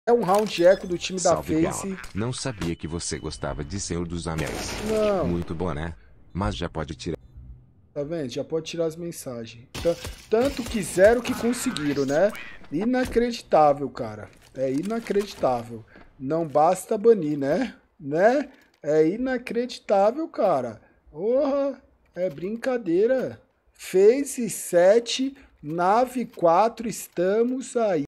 CONSEGUIRAM TILTAR O GAU COM ESSA MENSAGEM
Nós somos a tribo
A Tribo ajuda a Tribo

Clipe retirado das lives das próprias pessoas no vídeo.

Para todos os amantes de CS e aqueles que como eu são fãs do trabalho do grande homem que é o Gaules e de sua família Tribo Gaulesa.

"A vida é feita de perder os ecos e vencer os forçados" - Gaules